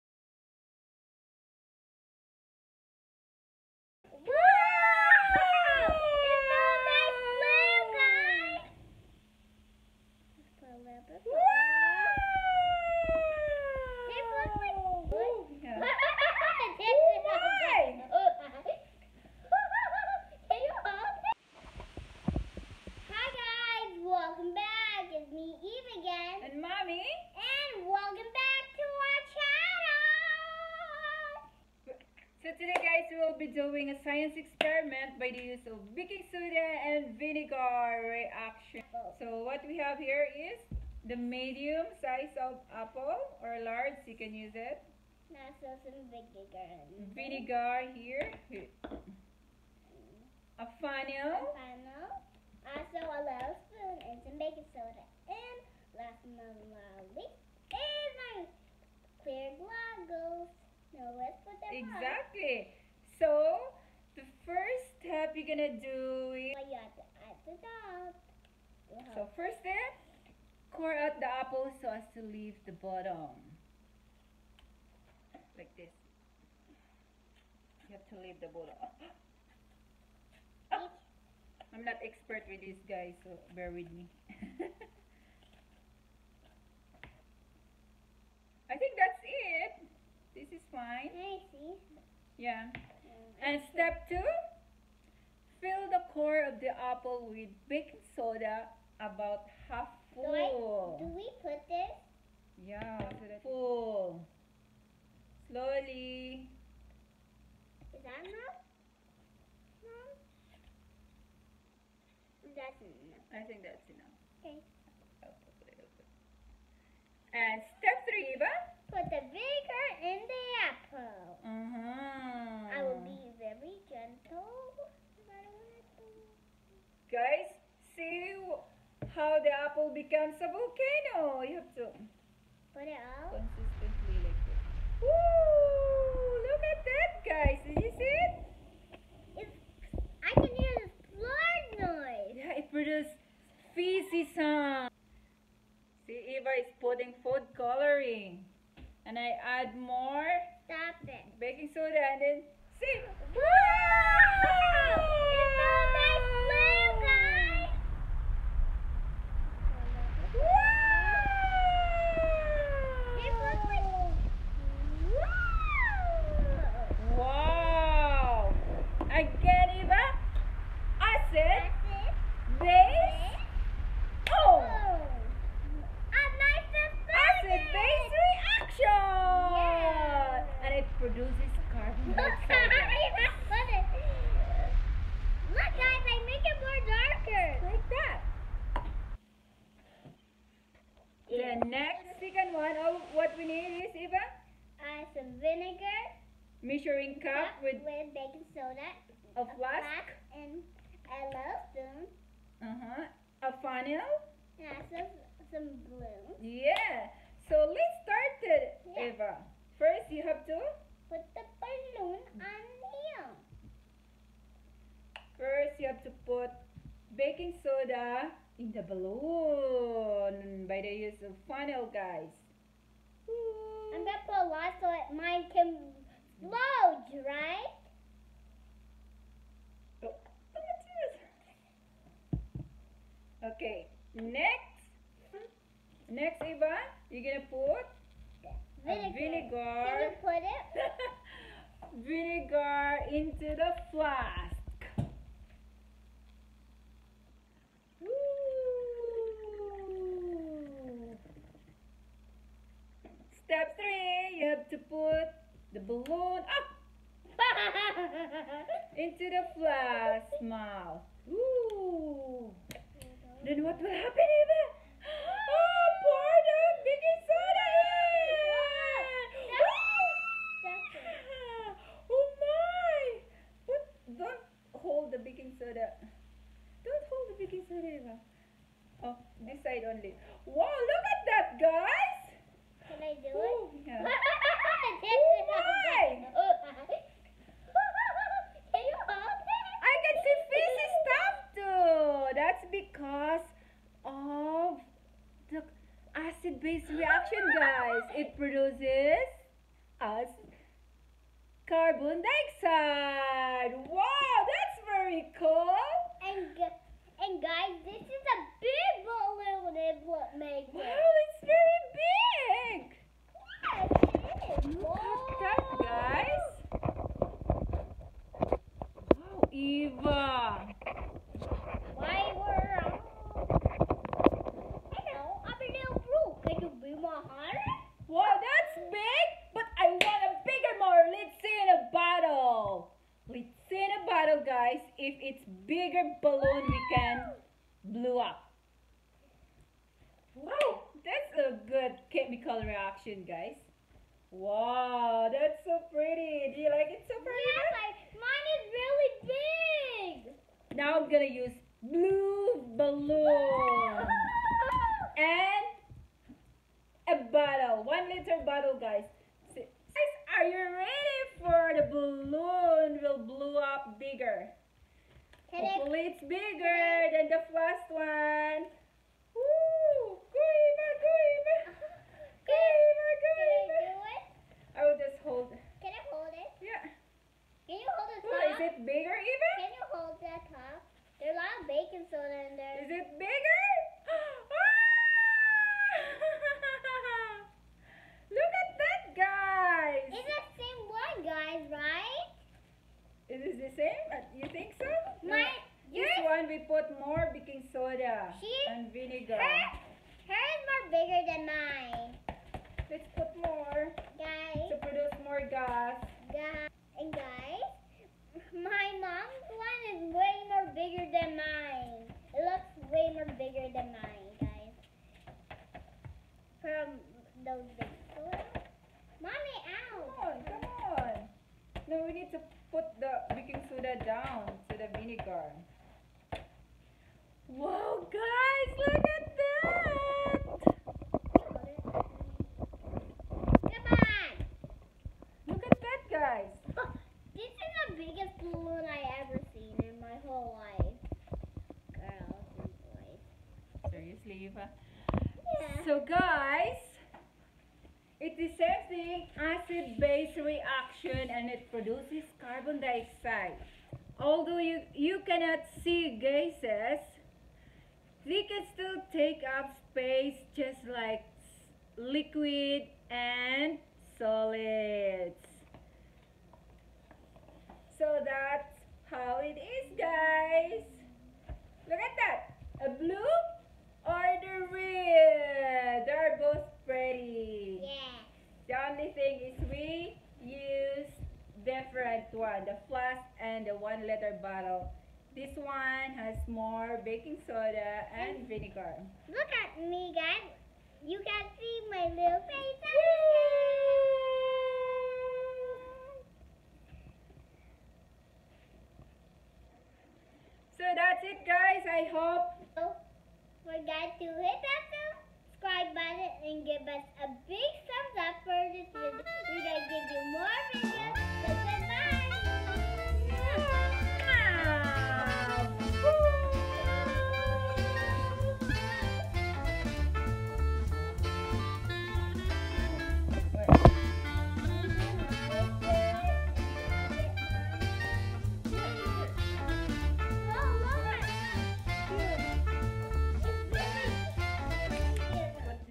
Baking Soda and Vinegar Reaction Science experiment # learn at home | Evadriel's world
Easy Science experiment that you can do at home by the use of baking soda and vinegar.